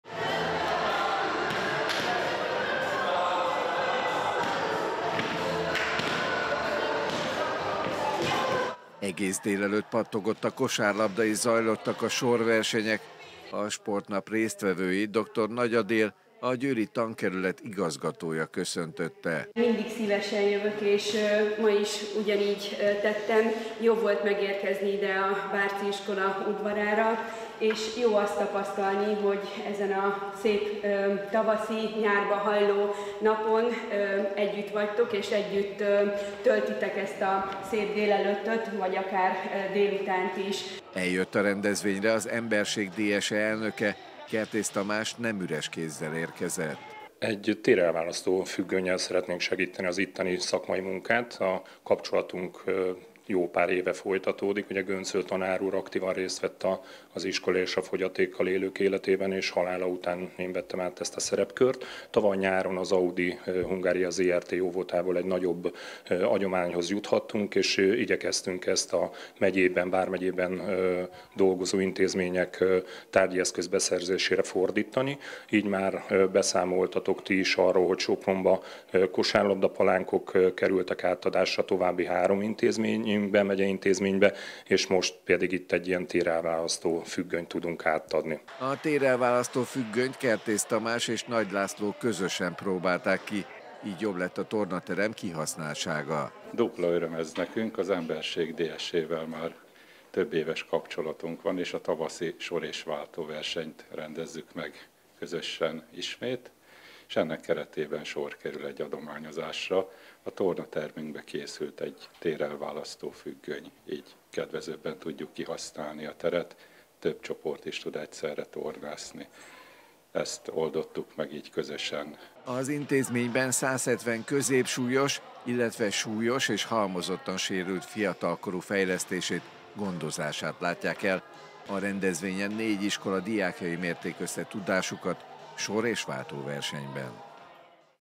Sportnap és adományozás a Bárczi-iskolában
Sportnapot tartottak a Bárczi Gusztáv Iskolában. Az intézményben mintegy 170 középsúlyos, illetve súlyos és halmozottan sérült fiatalkorú fejlesztését, gondozását látják el. A rendezvényen 4 iskola diákjai mérték össze tudásukat sor- és váltóversenyben. A sportnapon egy különleges ajándékot kapott az iskola. Az Emberség DSE egy térelválasztó függönyt ajándékozott a Bárczisoknak, amit a tornateremben tudnak majd hasznosítani.